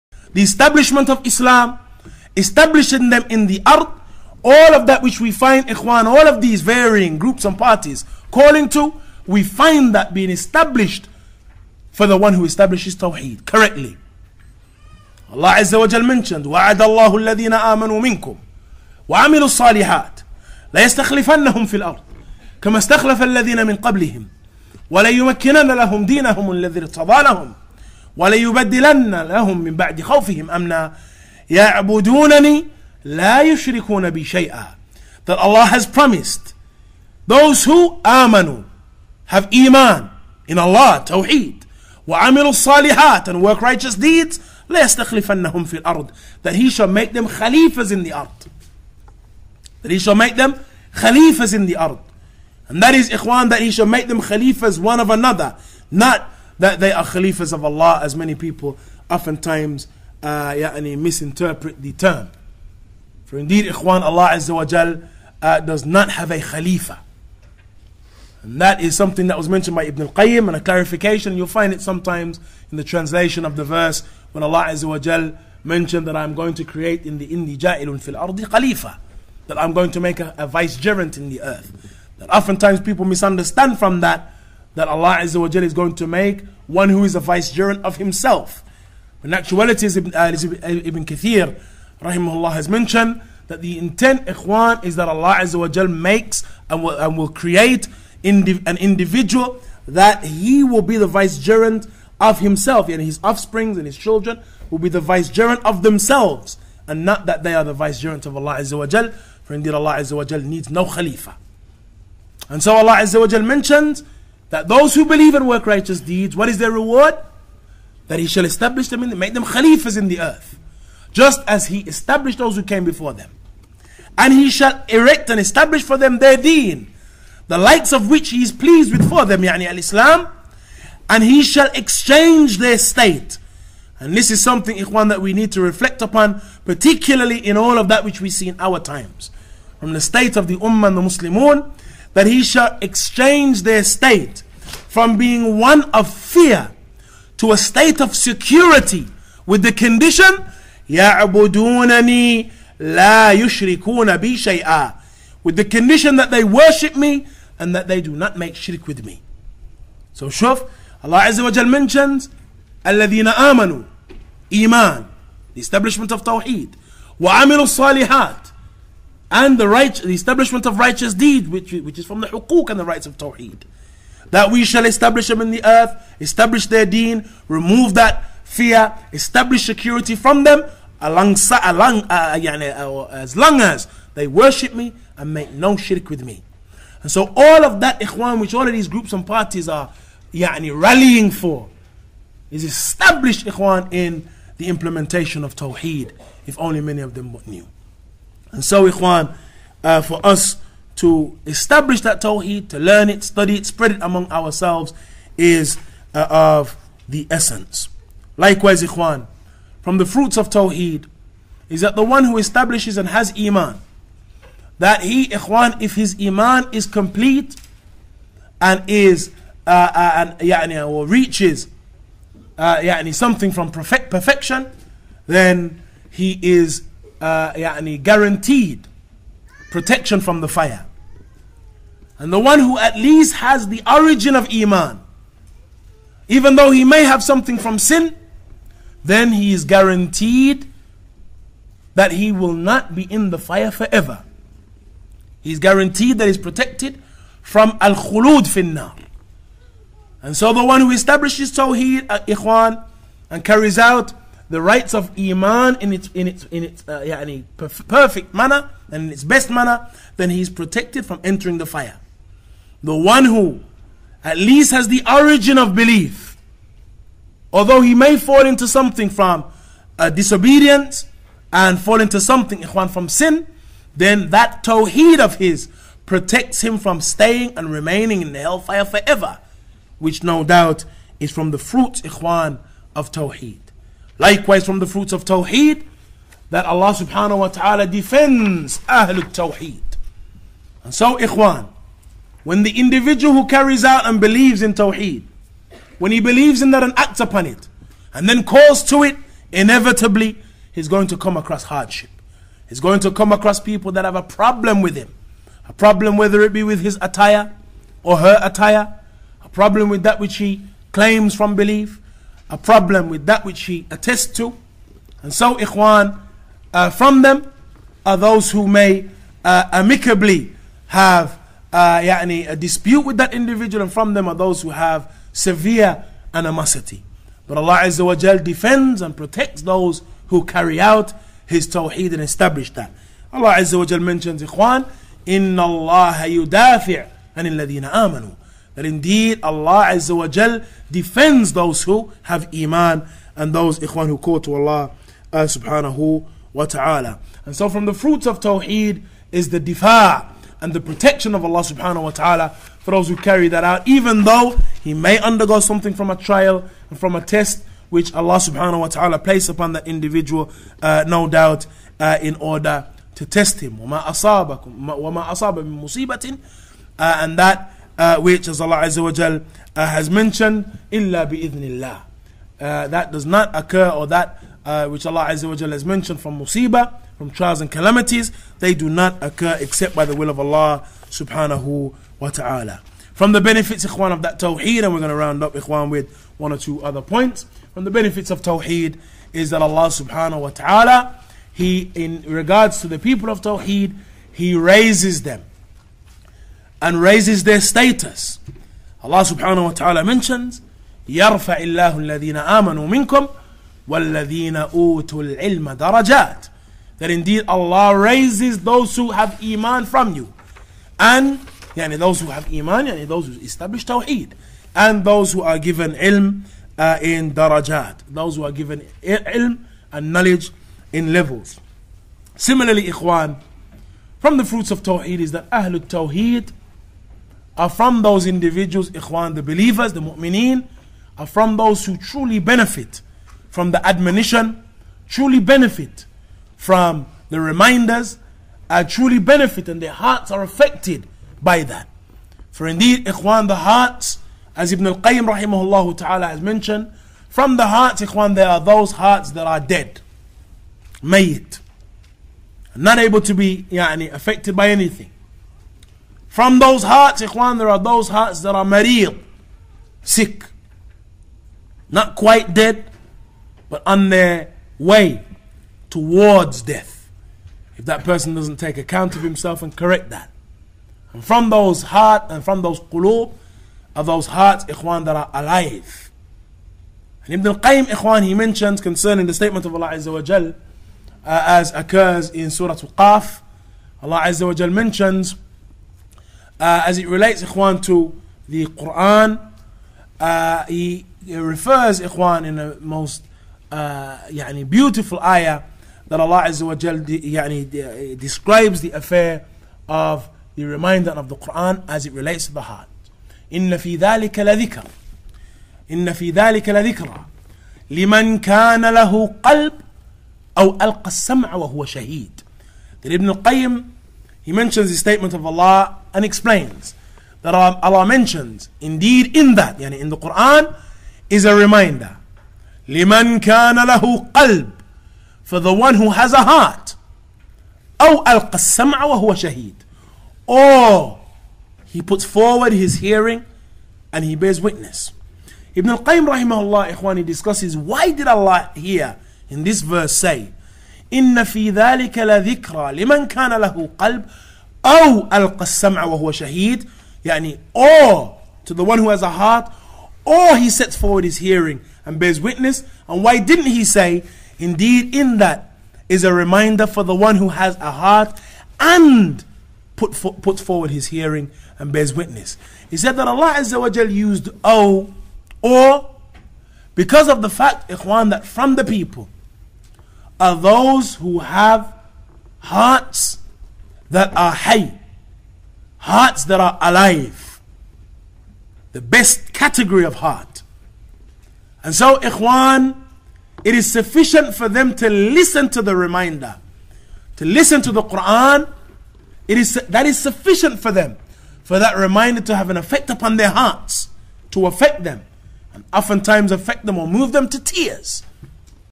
Monotheism (Tawheed) is the Key to Paradise Part 7
Tawheed (Monotheism) is the Key to Jannah (Garden of Eden; Paradise) by: Abu Hakeem Bilaal Davis by: Abu Hakeem Bilaal Davis